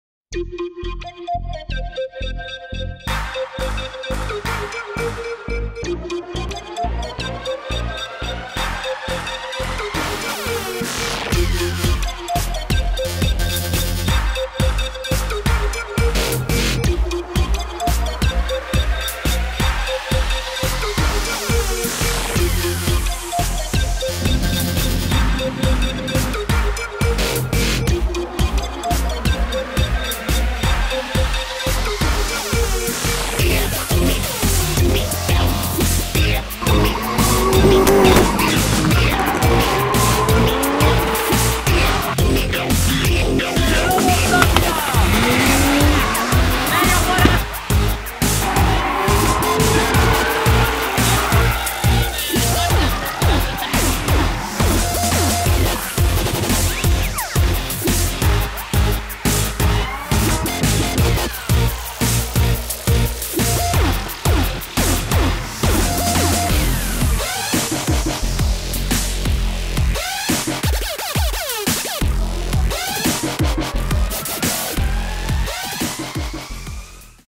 Campionatul National de Drift 2017 - rezumat Etapa Nr. 1
Prima etapa din Campionatul National de Drift 2017 s-a disputat pe circuitul de la Mariapocs, Ungaria, si a avut la start nu mai putin de 80 de piloti din 6 tari. Cel mai bine clasat pilot tricolor a fost Rares Girda, care a incheiat pe pozitia secunda.